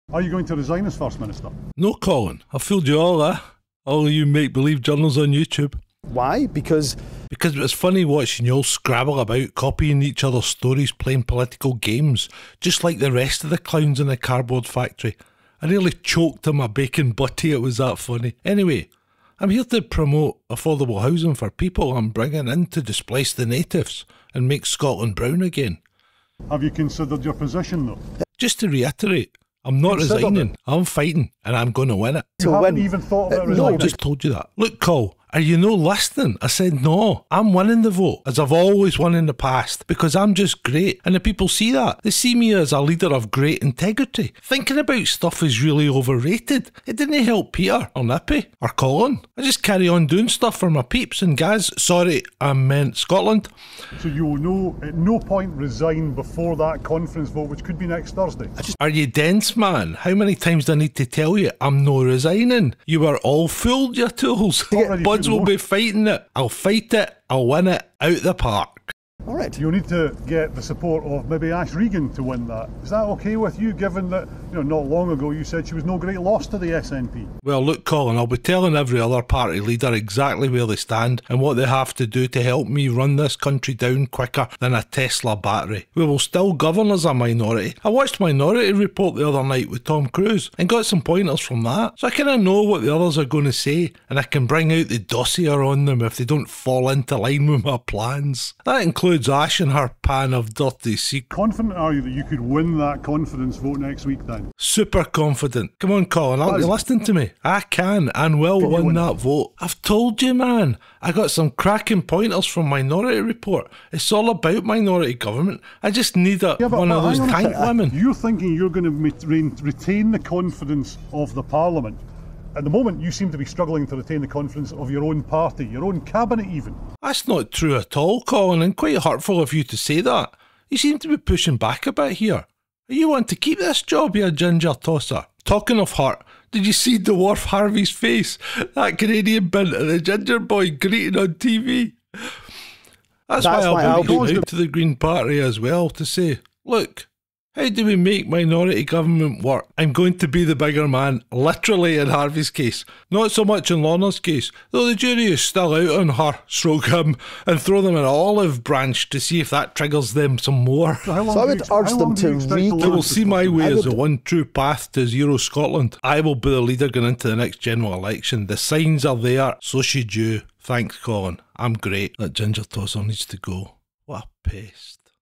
Himza Minority Report The SECRET WEAPON
Himza revels in tricking ALL the media into thinking he would resign, this guys ego is so big it has it's own bedroom. This is political satire.